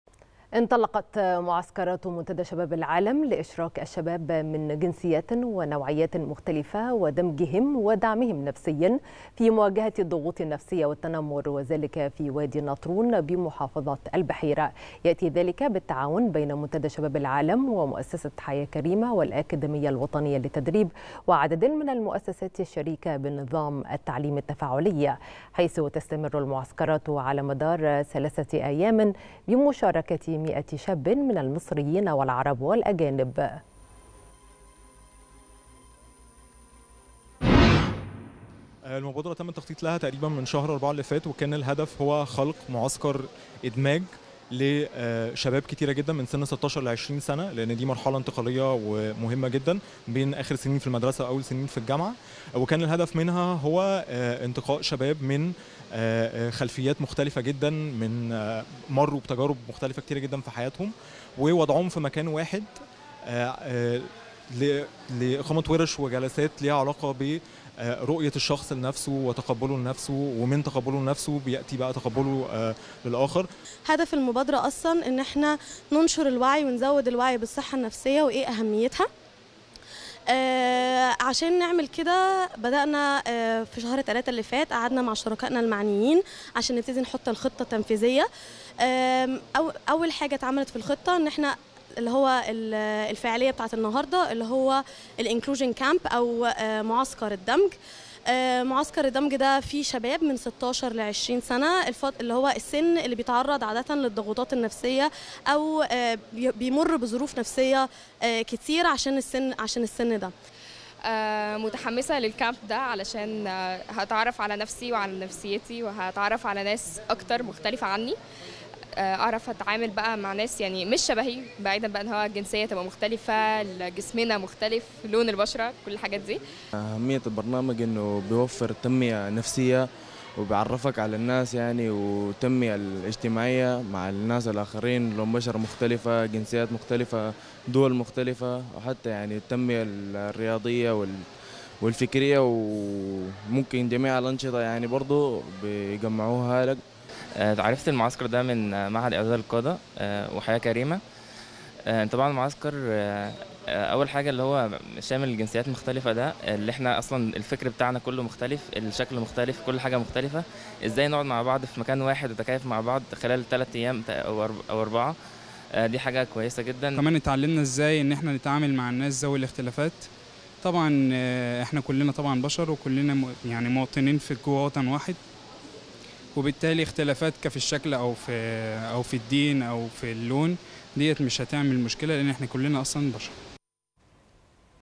"منتدى شباب العالم".. انطلاق معسكرات لإشراك الشباب من جنسيات مختلفة ودمجهم ودعمهم نفسيا
نشرة الثالثة | "منتدى شباب العالم".. انطلاق معسكرات لإشراك الشباب من جنسيات مختلفة ودمجهم ودعمهم نفسيا